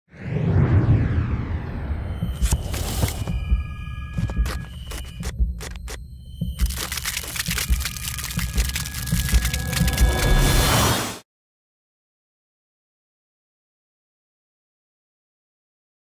BEI Cube Logo
This is a Cube Intro with a white background logo you can see a transparent Cube Intro on this channel & you will see the difference.
Dan Strobel